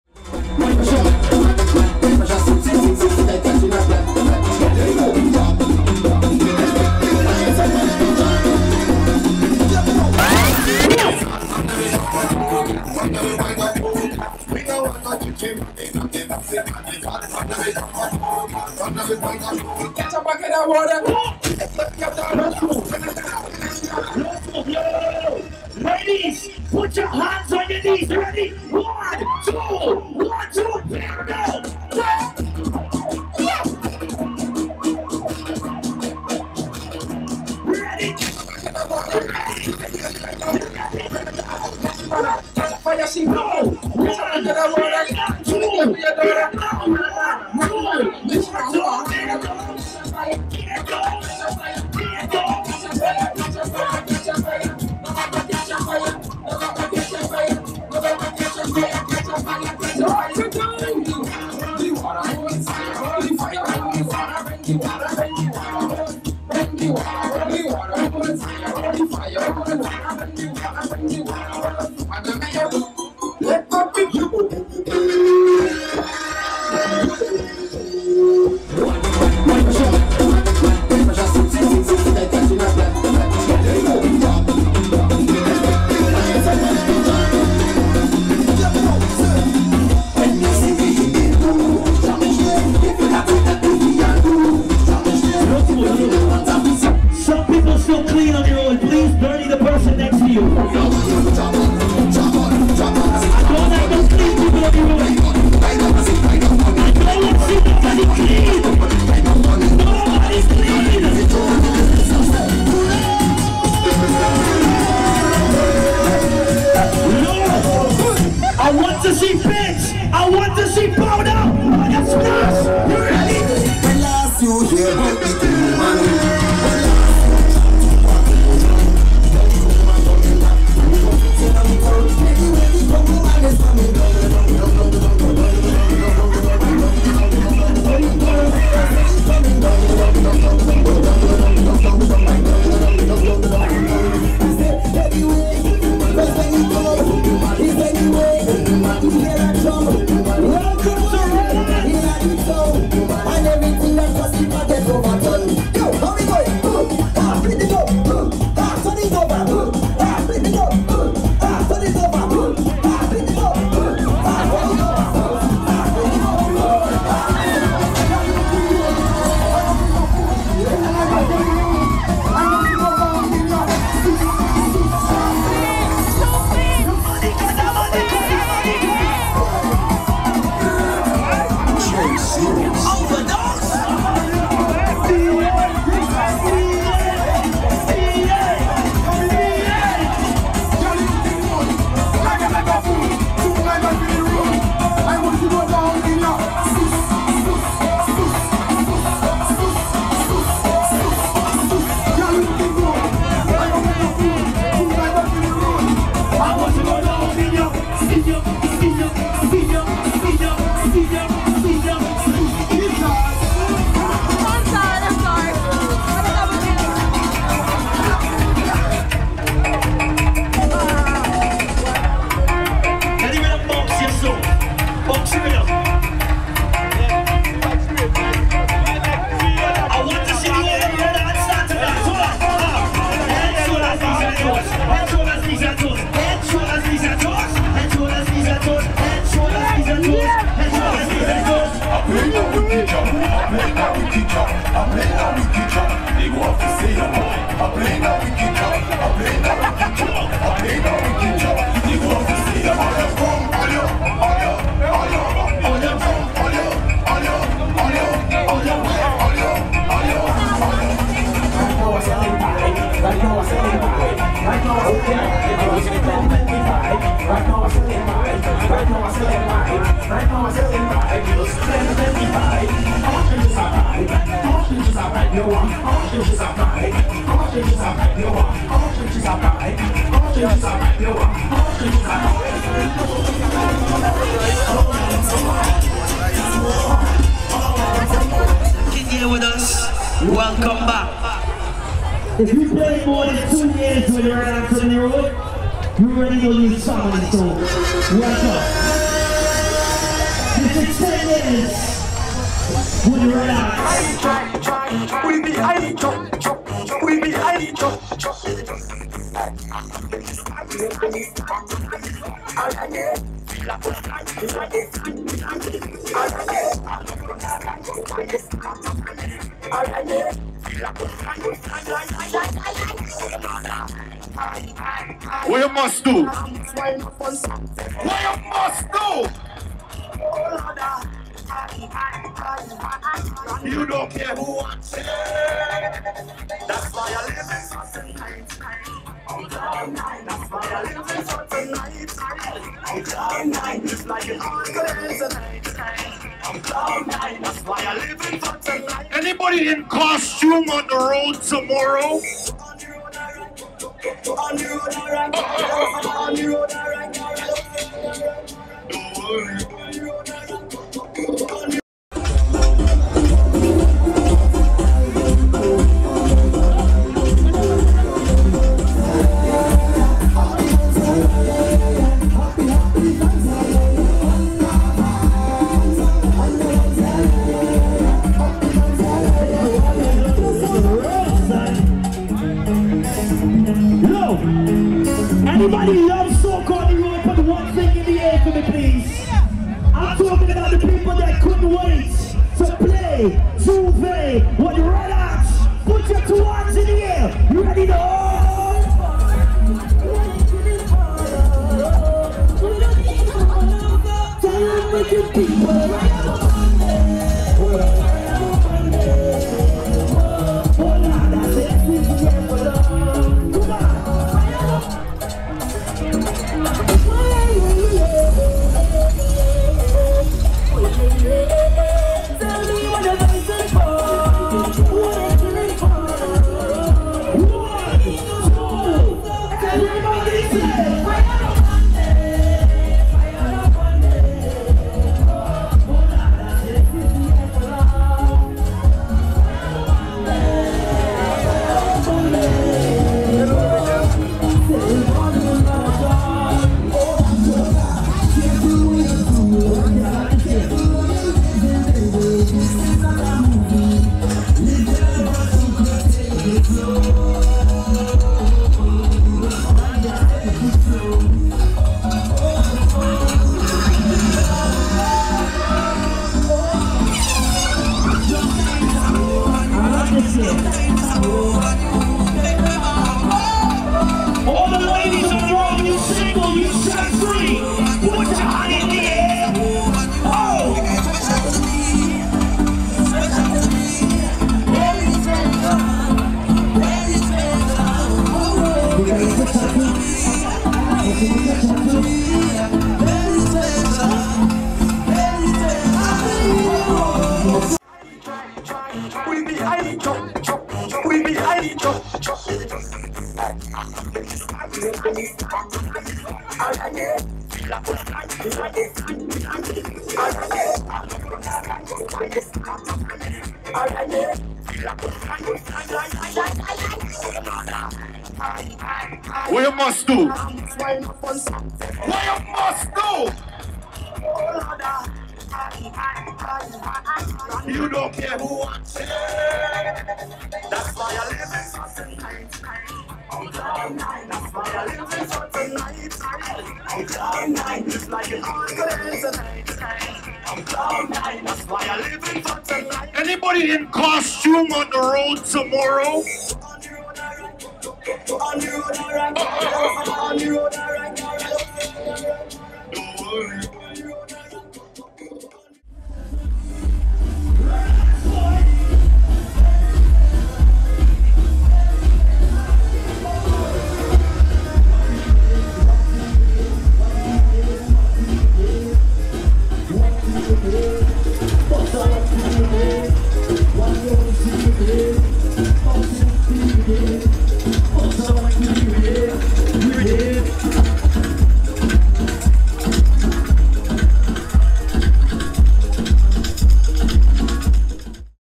A slice of the Caribbean in Miami, Action packed Miami Carnival 2022 Jouvert
Crazy Miami Jouvert experience, we got up close and personal for this video. Lots of action hope you guys appreciate it.